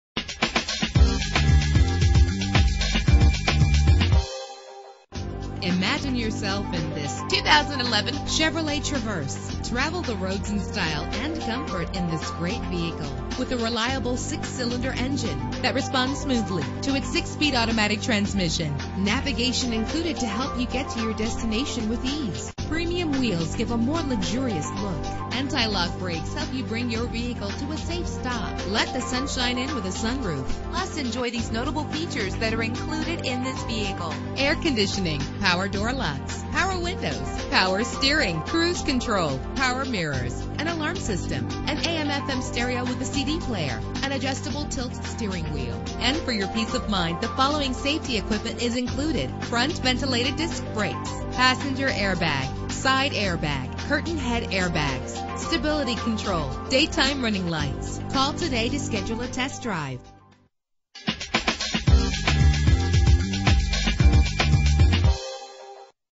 2011 Chevrolet Traverse - Costa Mesa CA
Year: 2011
Make: Chevrolet
Model: Traverse
Trim: 1LT
Engine: 3.6 liter 6 cylinder 24 valve
Transmission: 6-Speed Automatic
Color: Gray
Mileage: 3
Address:
2828 Harbor Blvd